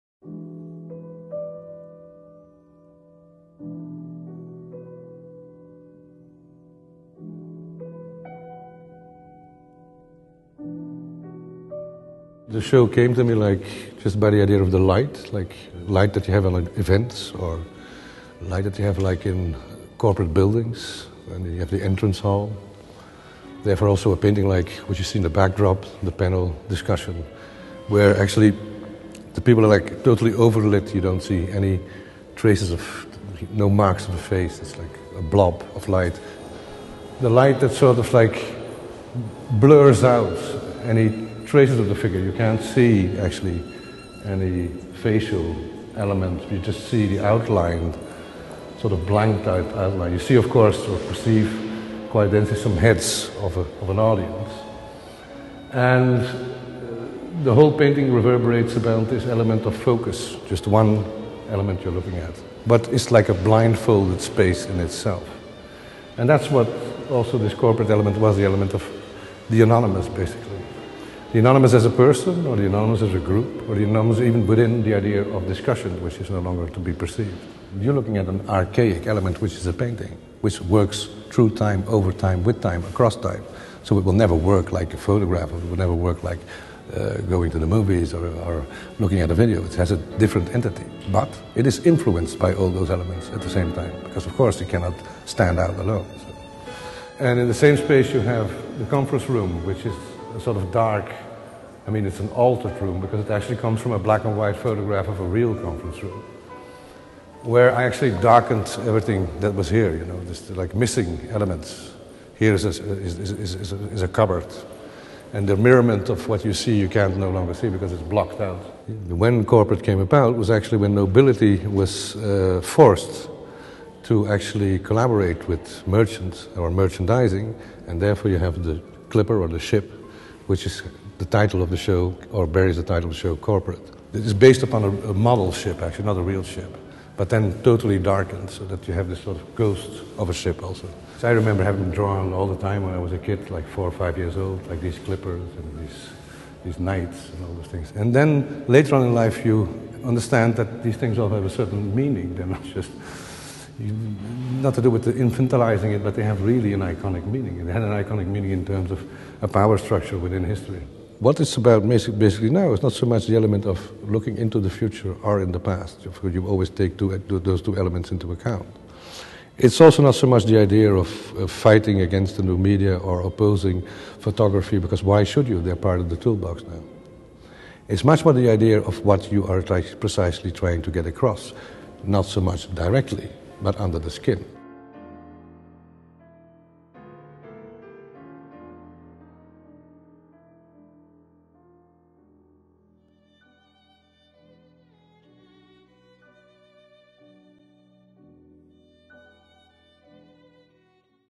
Luc Tuymans reflects on his new paintings
Luc Tuymans' latest body of work is currently on show at the David Zwirner Gallery in New York (until 21 December). Luc Tuymans spoke to Phaidon.com about the idea of the show which 'came to me through the idea of light: light that you have at events or in corporate buildings - in the entrance hall' and discusses some of his paintings on show in this video.